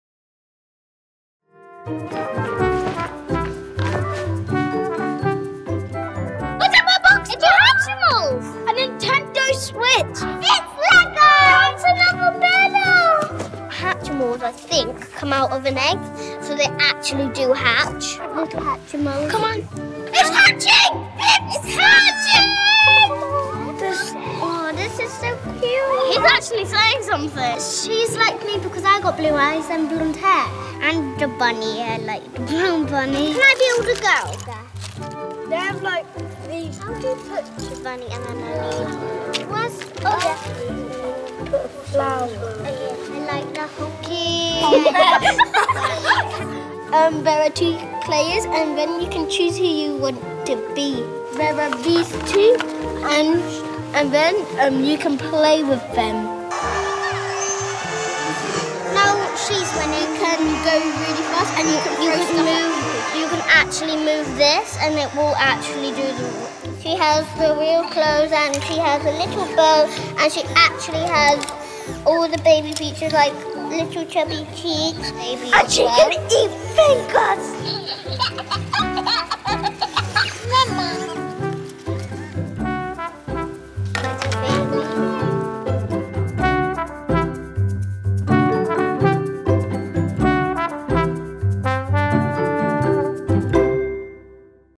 Christmas Gift Inspiration with... The Kids
Looking for Christmas gift help or inspiration for children? Meet... The Kids - our very own present testers! 🎁

To make it even easier to choose the perfect gifts this Christmas, we’ve enlisted four elite teams to see what they make of our top products...

In this edition, it’s the kids!